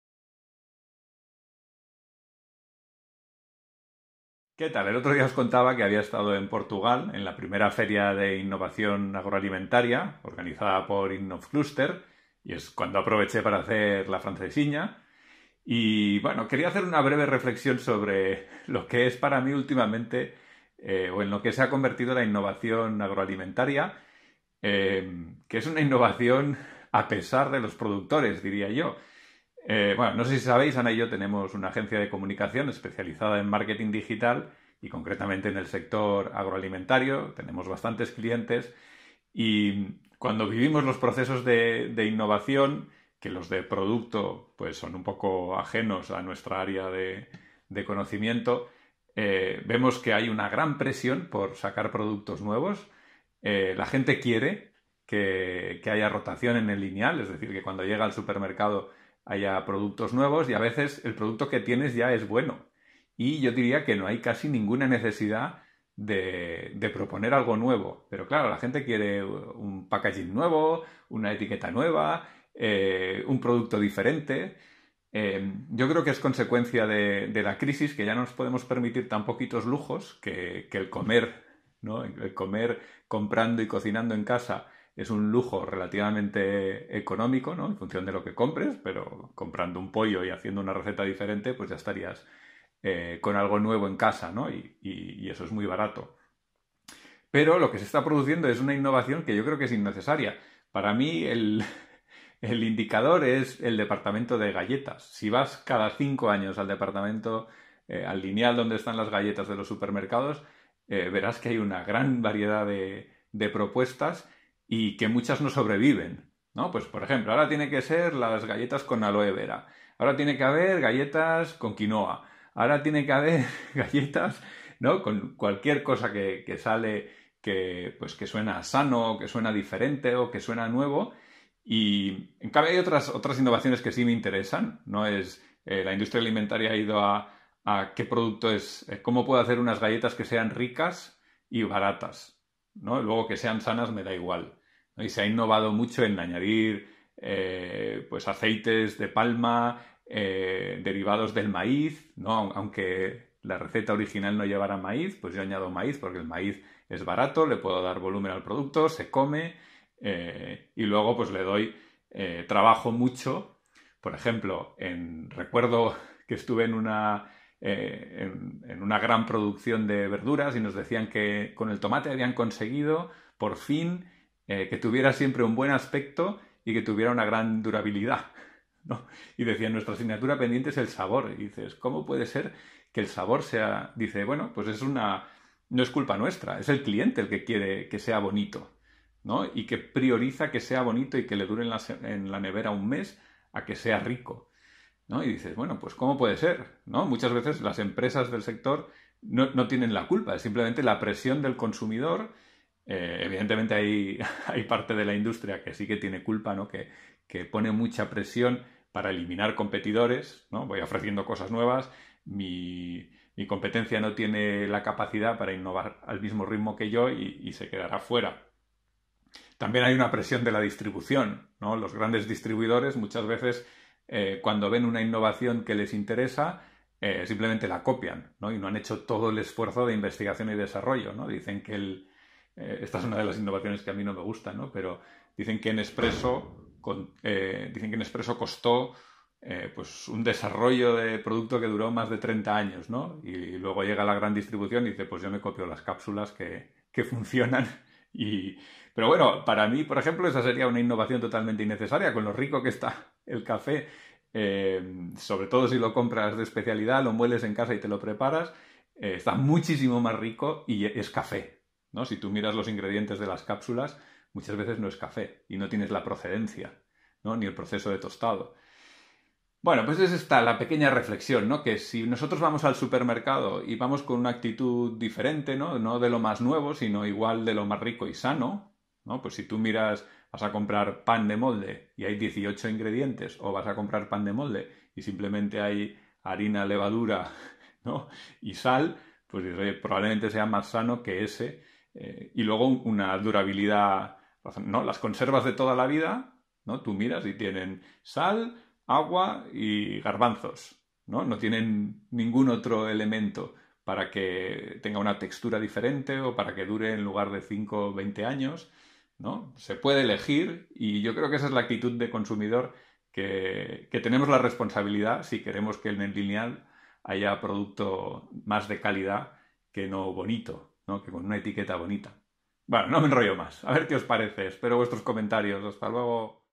SOBRE INNOVACIÓN AGROALIMENTARIA
Breve reflexión sobre los caminos que está tomando el sector agroalimentario con relación a la innovación. Es un comentario breve y algo superficial, pero con algunas ideas que creo interesantes.